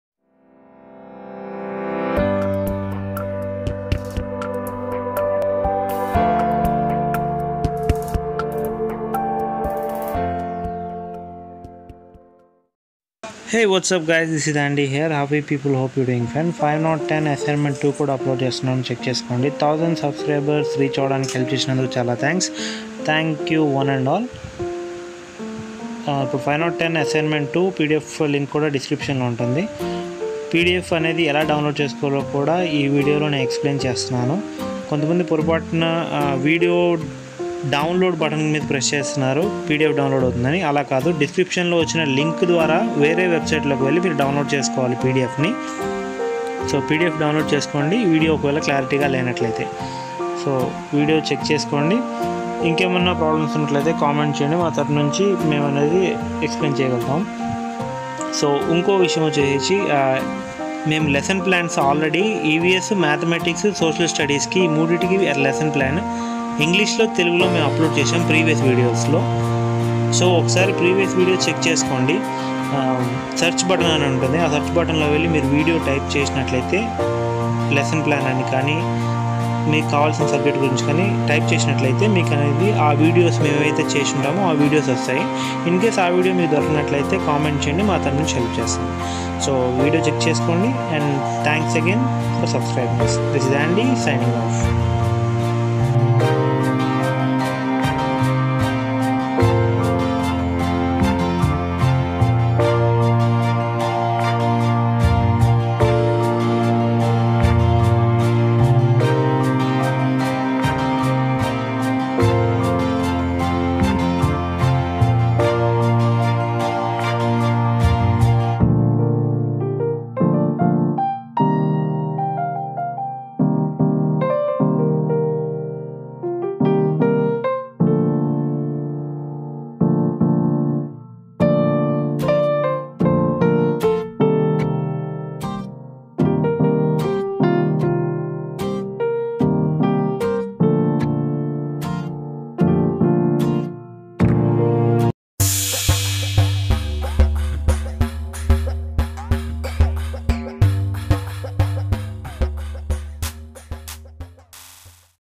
510 ASSIGNMENT -2 TELUGU
510 ASSIGNMENT-2 QUESTIONS
Assignment-2
1. Explain the following points with one example related to the Expository Transmission, Discovery and Inquiry approach of teaching science.
• Structured-ness Teaching
• Learning Process
OR
Select a topic of your choice from class VII science text book and analyze it whether it is based on theme or subject centered approach. Justify your answer also.